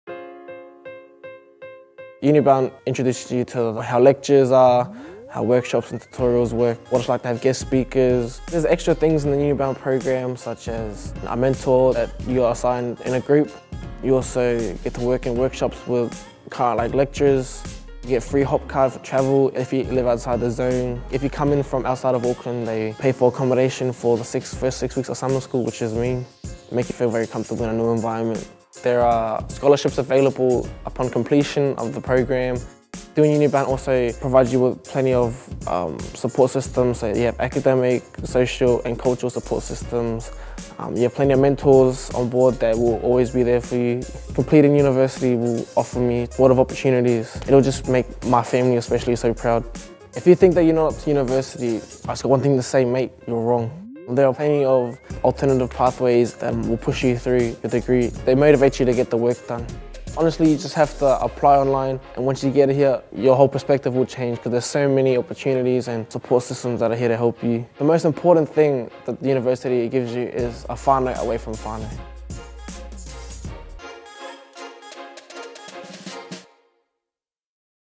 UniBound – Dion’s story
Hear from Dion Westrupp what it’s like to take part in the UniBound programme (Certificate for Academic Preparation).